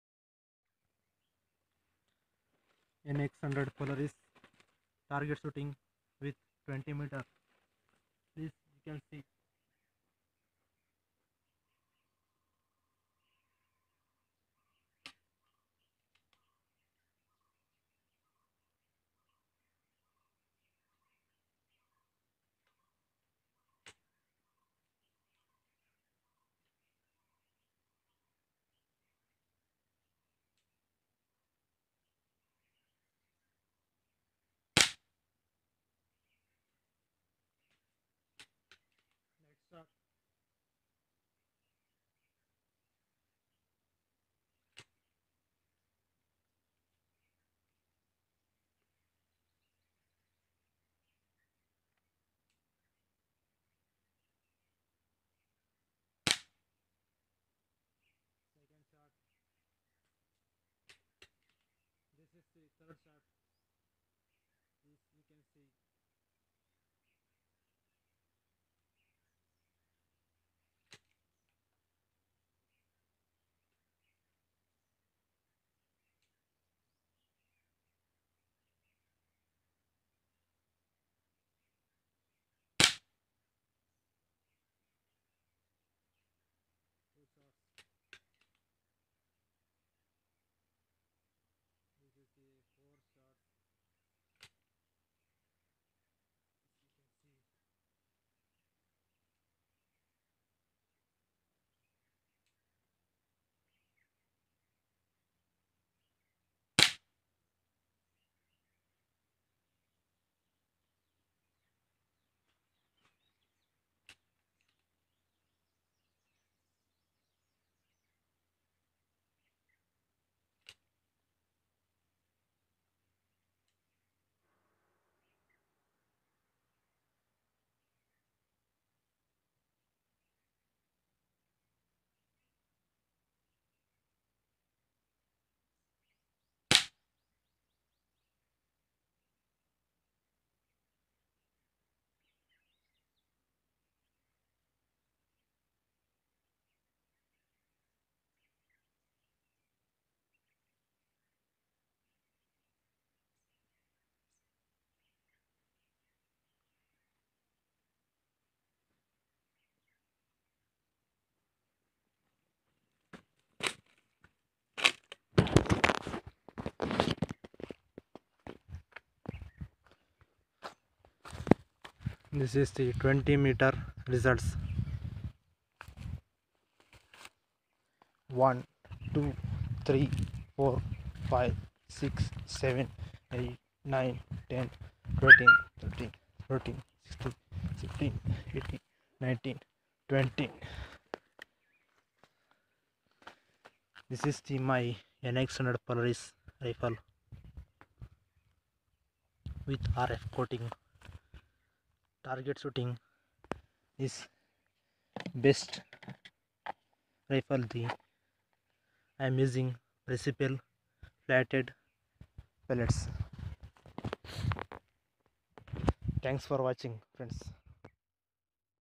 Nx100 Polaris .177 target 🎯 shooting at15 metar
Simply Stated : Air guns that emit energy less than 20 joules and has a caliber less than or equal to 0.177 will NOT need any license. That would mean an air gun that produces energy more than 20 joules and has a caliber more or equal to than 0.177 mm will need you to have a license. You can buy .177